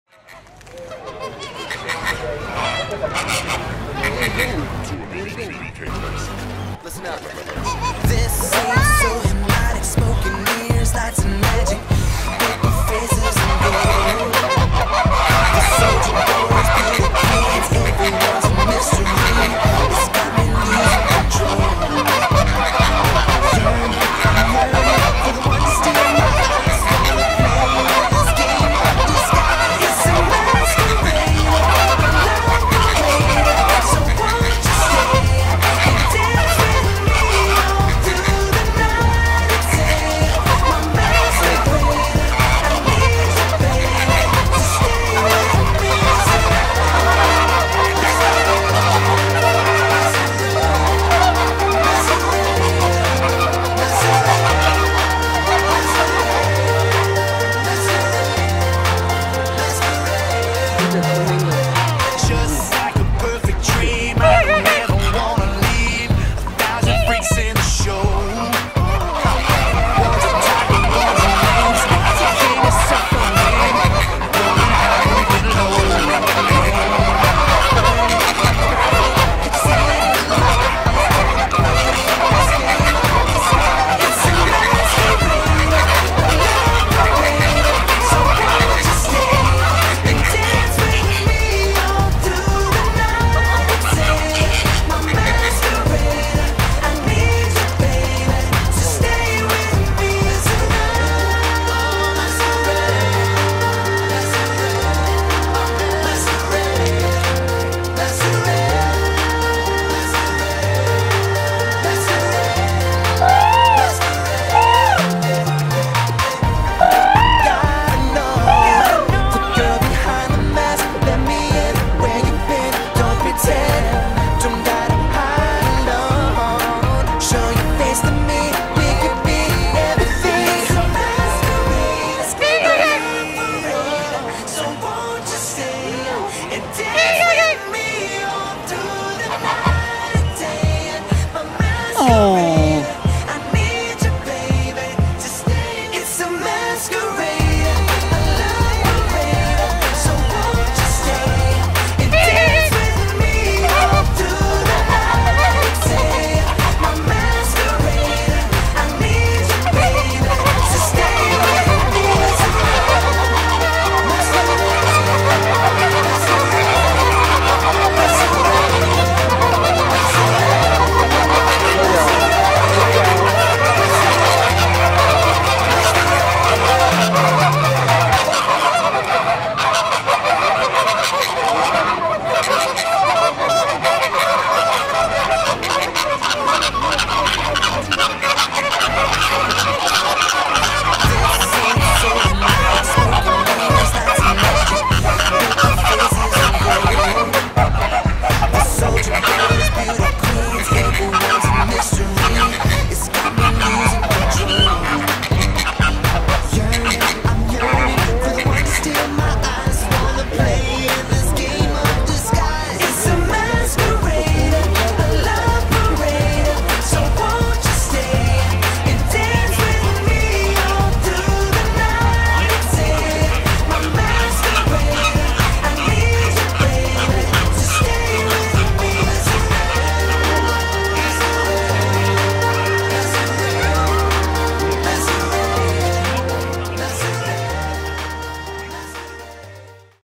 Flamingo Flash Mob
These flamingos display their wings and walk together. When one starts, the others do what that one is doing.